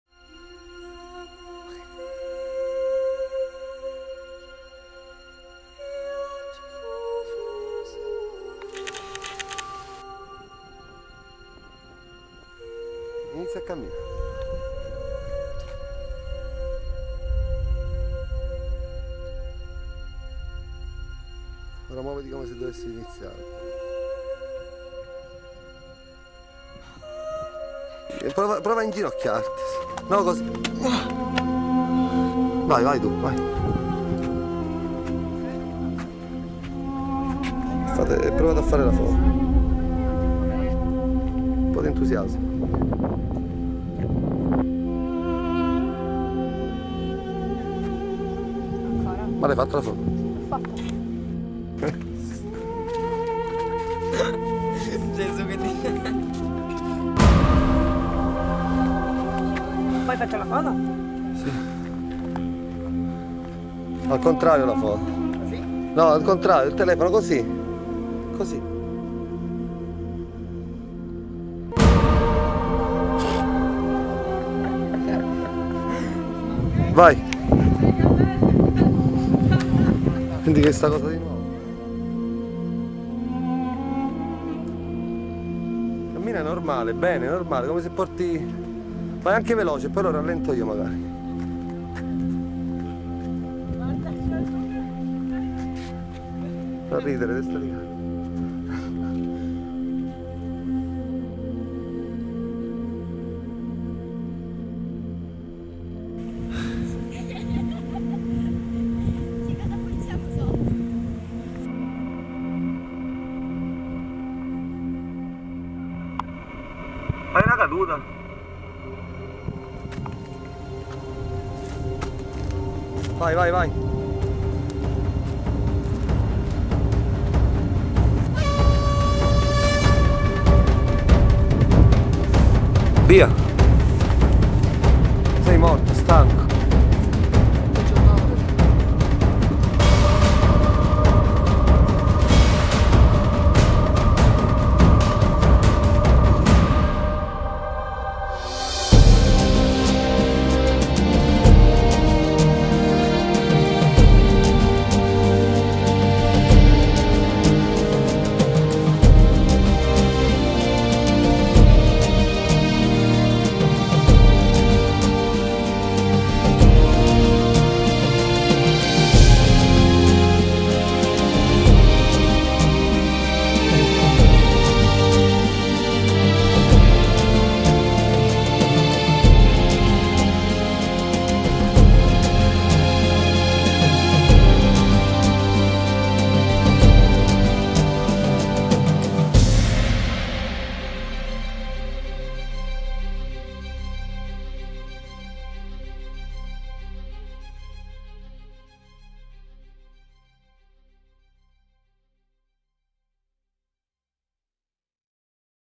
BackStage 2014 - Crucis Web® Short [Scene inedite]
Backstage 2014 - Alcuni momenti inediti tratti dal backstage di  - Crucis Web® - il cortometraggio che anticipa il progetto d'arte 2014 dell'astrattista Alessandro Giorgetti
Scritto e diretto da Alessandro Giorgetti
Backstage 2014